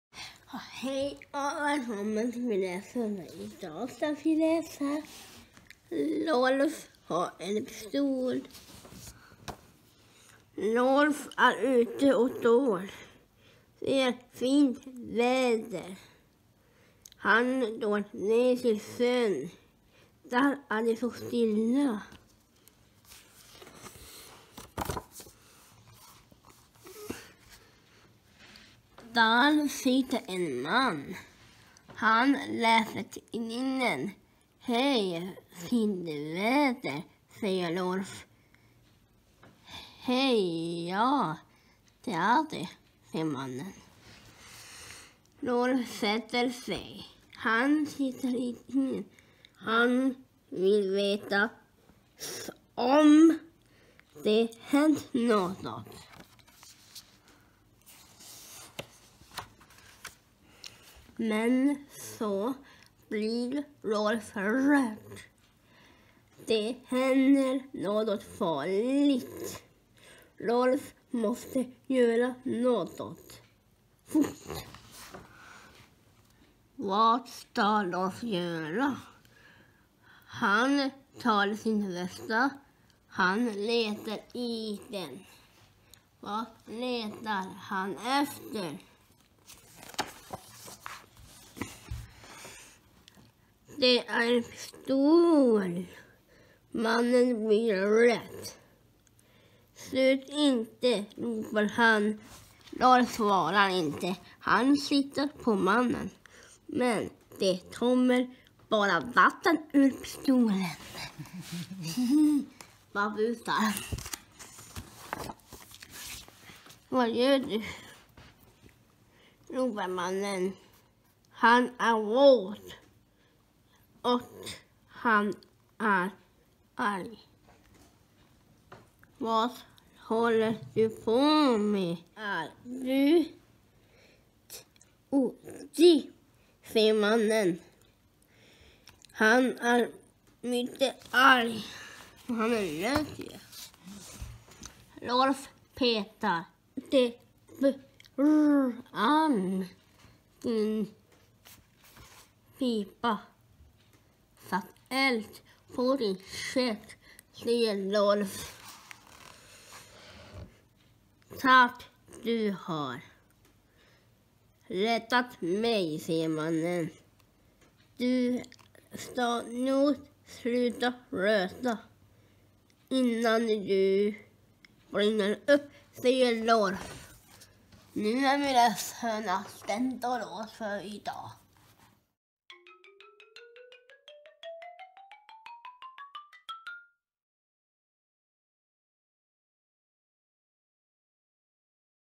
Max läser Rolf har en pistol
Max läser varje vecka en bok i sin läshörna.
Här läser han boken: "Rolf har en pistol".